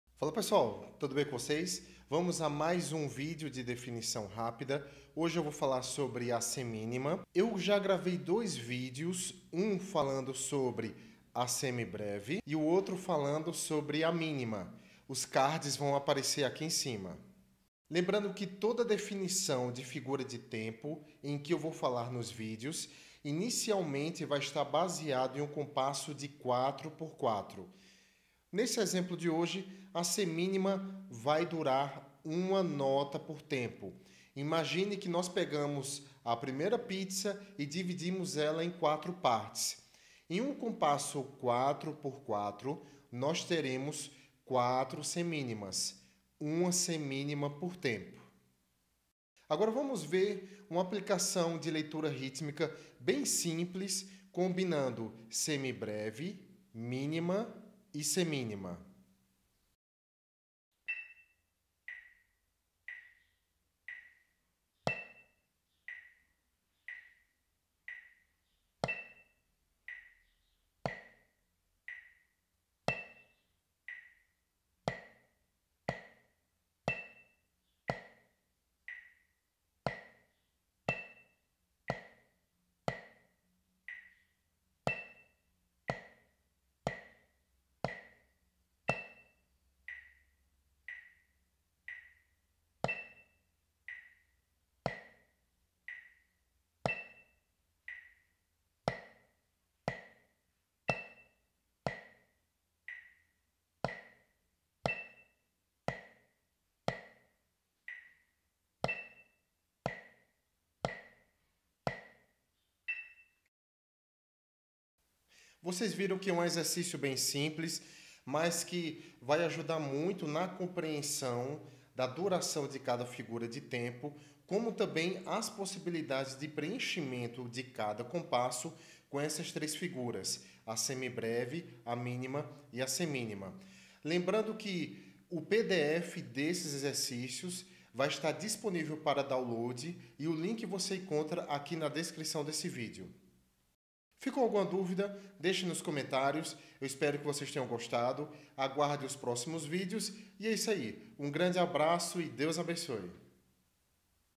Definição Rápida: SEMÍNIMA + Aplicação de Leitura Rítmica: Semibreve, Mínima e Semínima.
Hoje vou falar sobre a Semínima:

Para facilitar a compreensão da sua duração de tempo, vamos imaginar uma pizza dividida em 4 partes, assim é a divisão da Semínima em um compasso de 4(Quatro) tempos, com o denominado em 4 também, ou seja 4/4.

Resumidamente, teremos Uma nota por tempo.